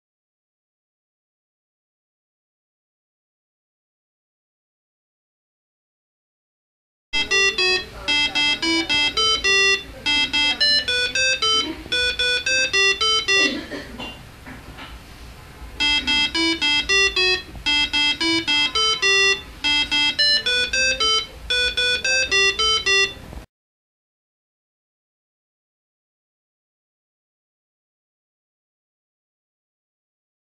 Playing Tones On MSP430 Launchpad
Play tones on MSP430 by porting the Arduino Library to MSP430.
This can help you make Happy Birthday Cards, Door Bells, Alarms and what not!